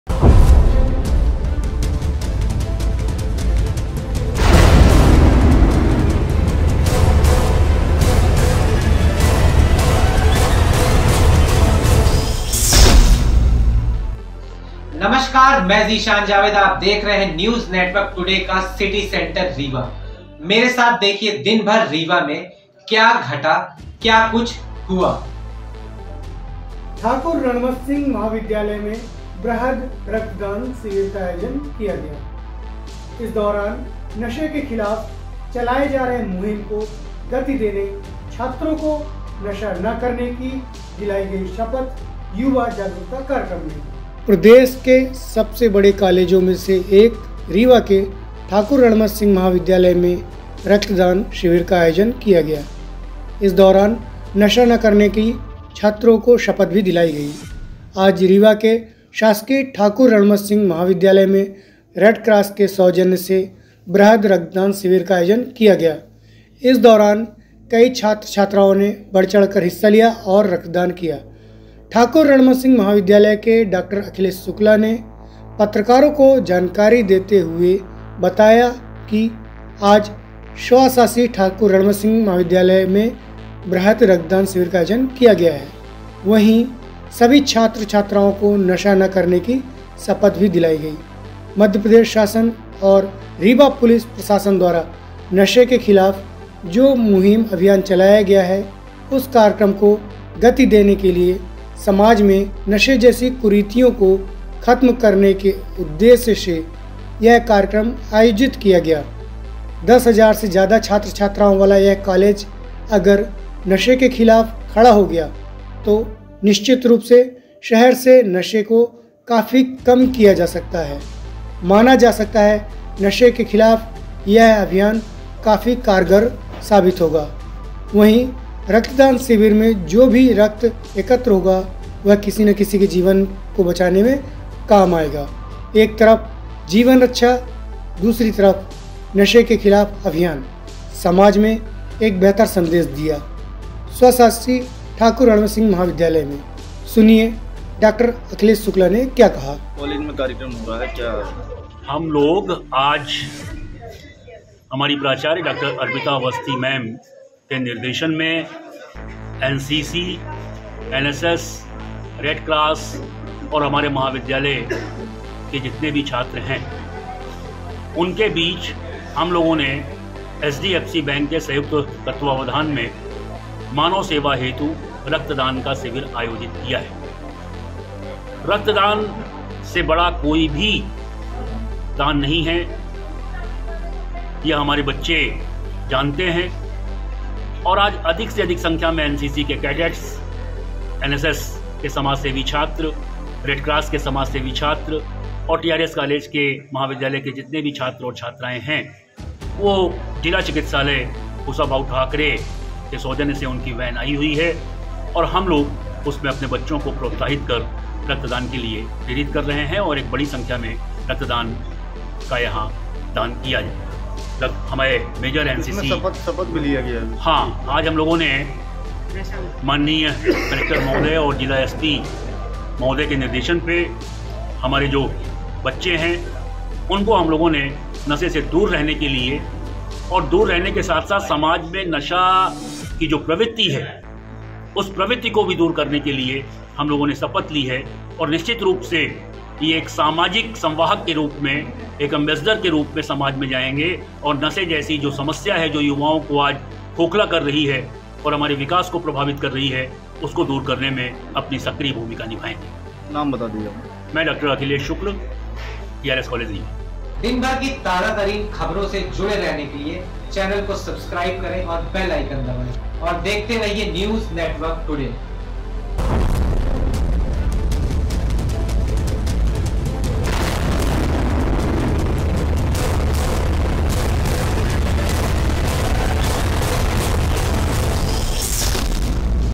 रीवा के ठाकुर रणमत सिंह महाविद्यालय में रक्तदान शिविर और नशे के खिलाफ शपथ का आयोजन किया गया ||NNT
रीवा के ठाकुर रणमत सिंह महाविद्यालय में रक्तदान शिविर और नशे के खिलाफ शपथ का आयोजन किया गया